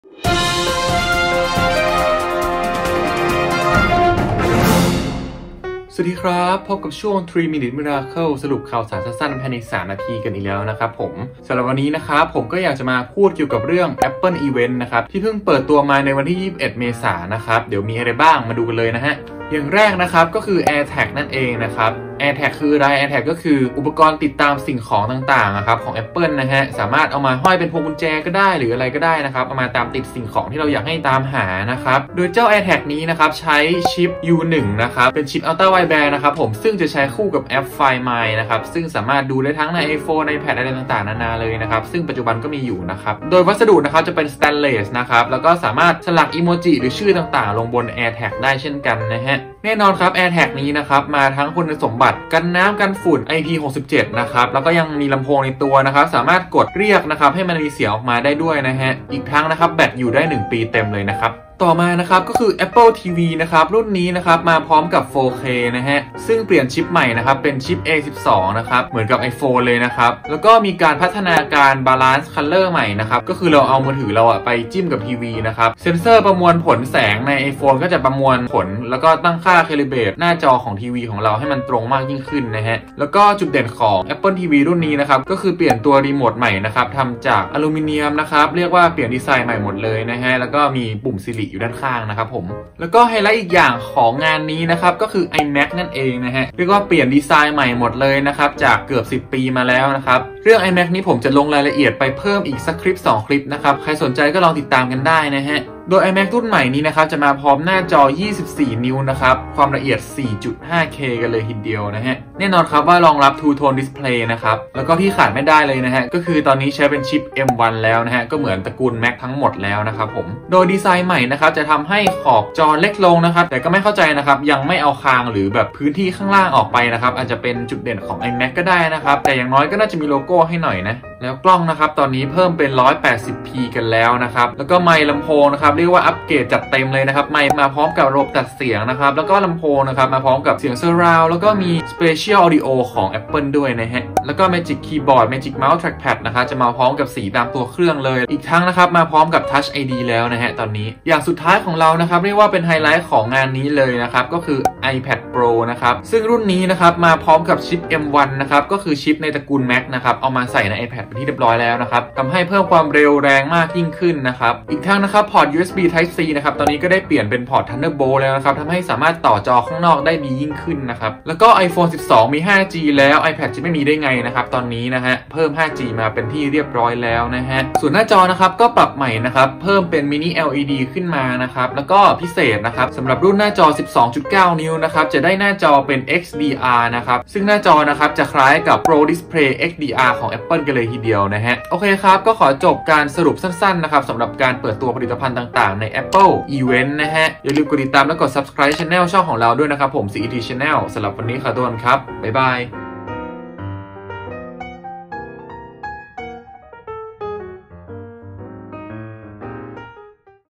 สรุปงาน Apple Event 2021 สั้นๆใน 3 นาที
0:00 intro
0:19 AirTag
1:00 Apple TV
1:35 iMac
2:34 iPad Pro